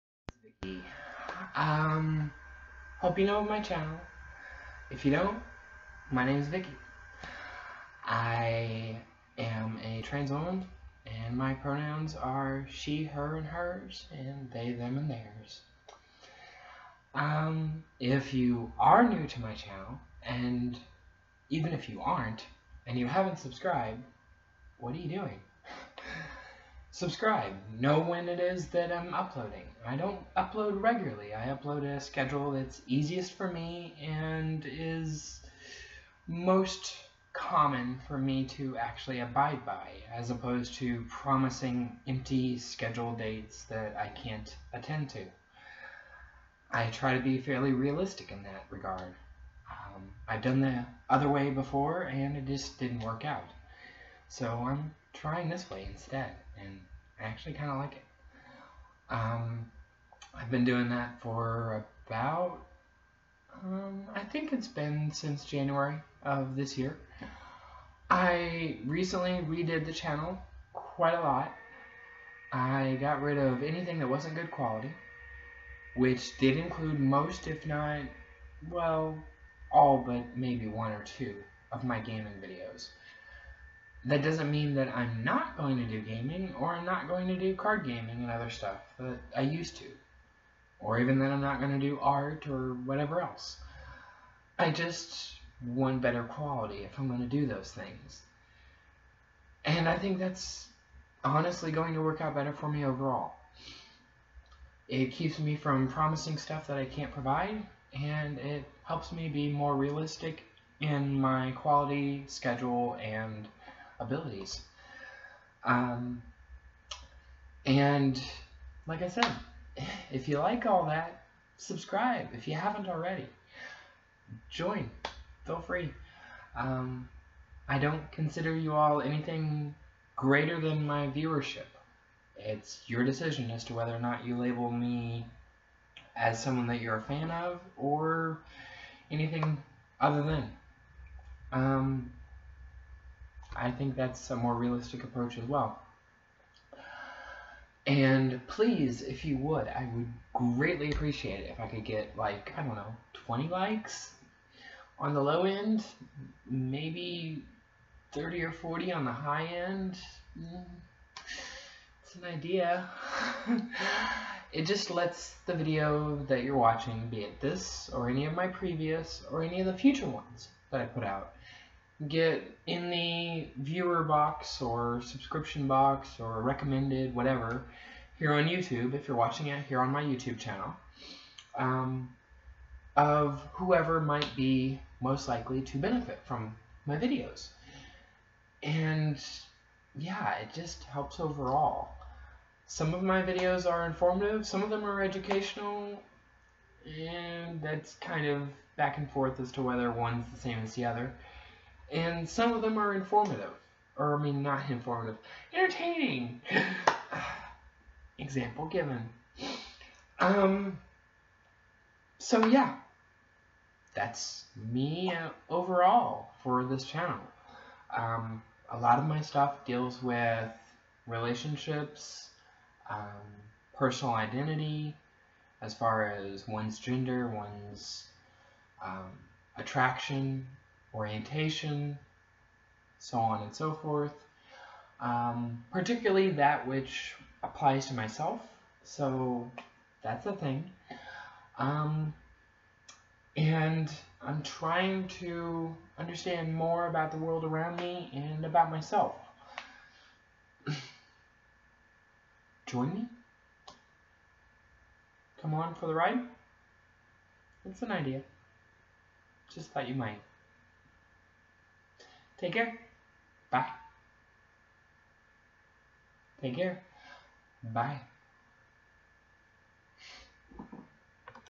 Welcome to My Channel | Vikktoria Leighbhair
=[Plz Read Description Below for Info]=
{About Me}
I am Vikki Alessandras Leighbhair [Mark Vincent Oberfirst] a.k.a. "UltimateJudgement". I am a transwoman born in 1980. I am a pansexual and polyamorous. My philosophical stances are naturist, atheist and transhumanist. I am an avid game enthusiast typically. I began this channel in September of 2009 and began uploading in October 2009.
I hope you enjoy my SolemnOfferings I post. I have no currently regularized uploading schedule and do so as I am able. I happily accept suggestions, messages and constructive criticism. Stay Supaa Sugoi.
Be Well